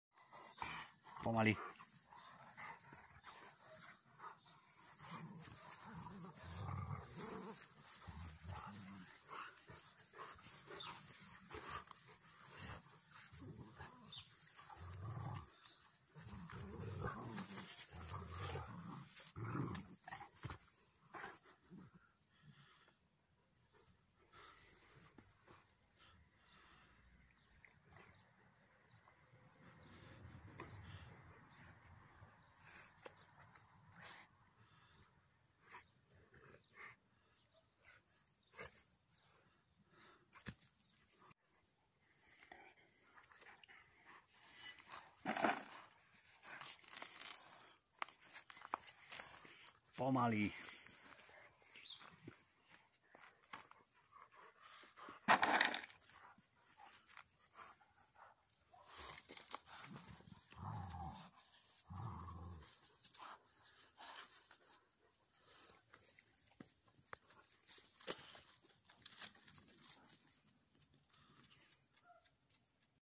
Rottweilers - WAR OF TITANS :-)
ARES - 24 months, 54 kg/ verzus/ HECTOR - 7 weeks, 6 kg
(Falon Ares Marol May/vs/Hector Troy Marol - May)